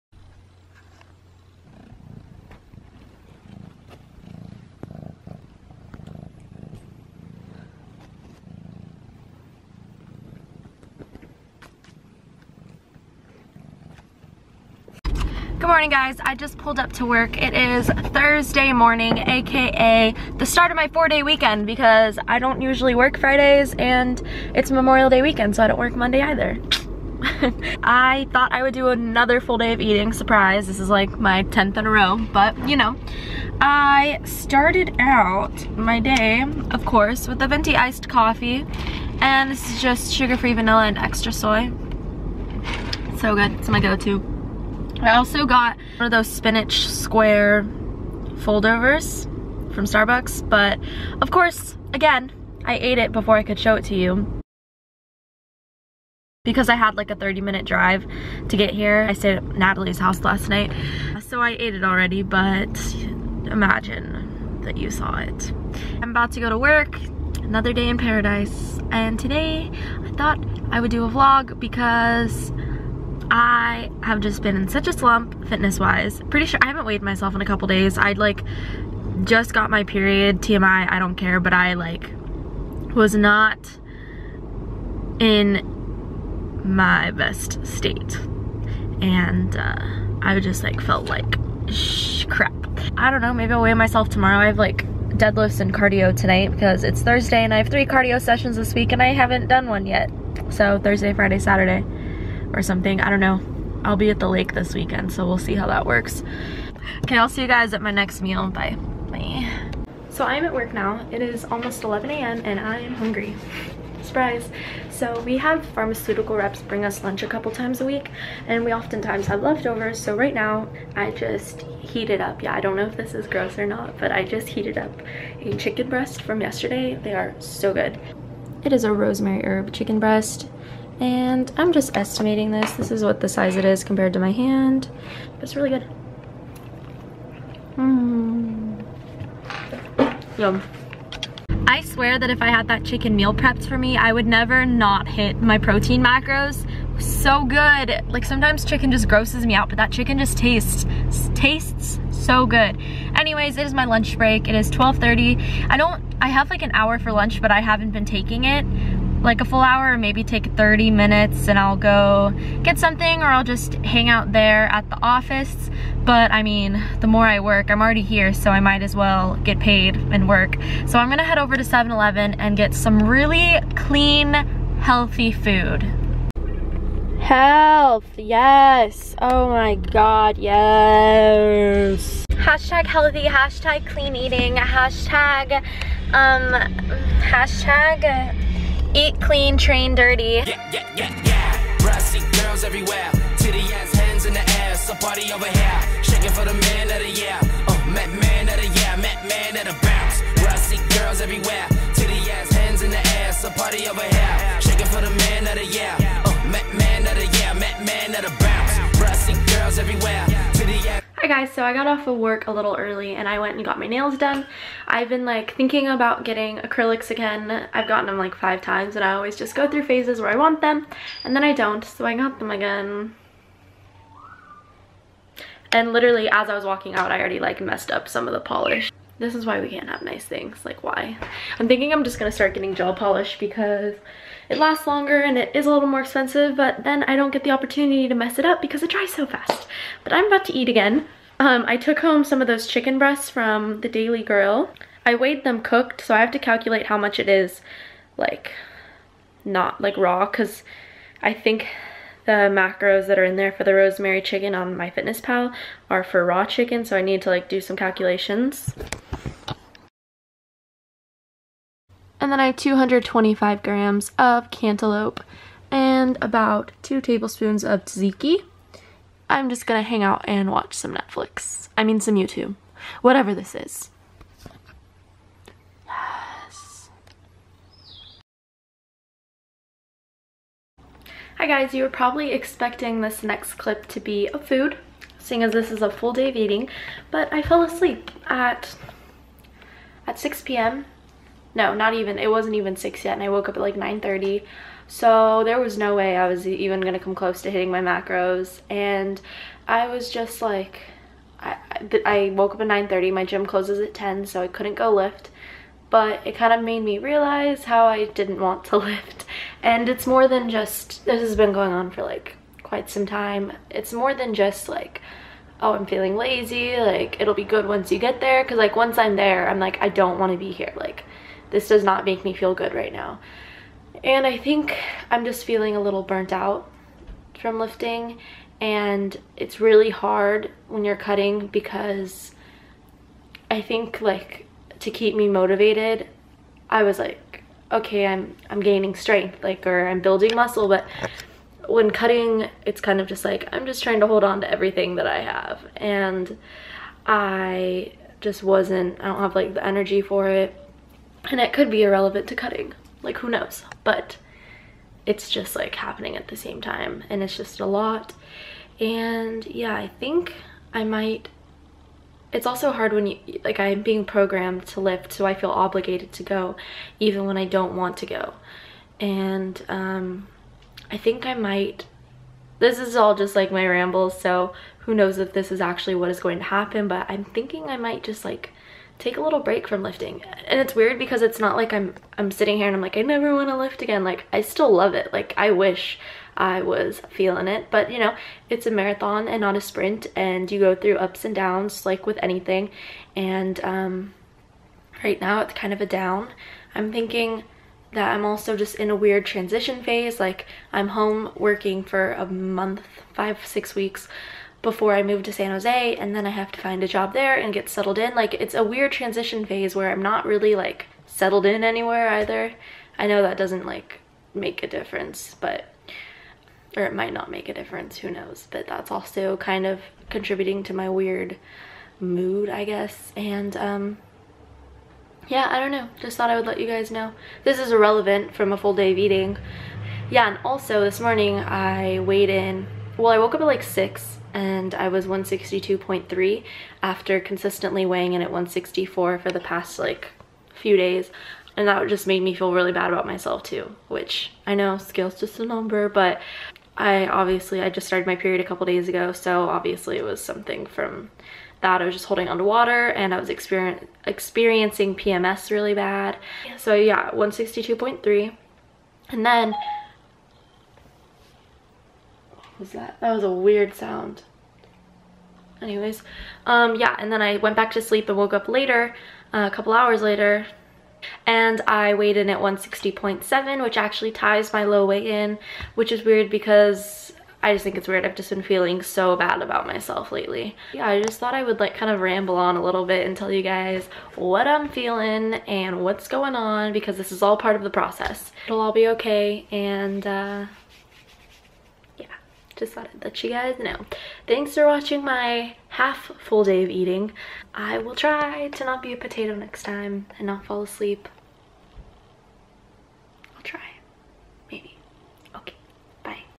Feeling Burnt Out? | College Cut Ep. 8
5/25/16
It has been about a week and a half since I filmed this, and I am happy to say that I am moving in a positive direction with lifting! I will explain what's happening a couple vlogs from now.

Not everyday is perfect, this journey isn't linear, and everyone gets in ruts! Stick to it, you'll get through it. :)

PERSONAL INSTAGRAM: @LPdawg_
FITNESS INSTAGRAM: @LPlifts_

See you in my next video :) xoxo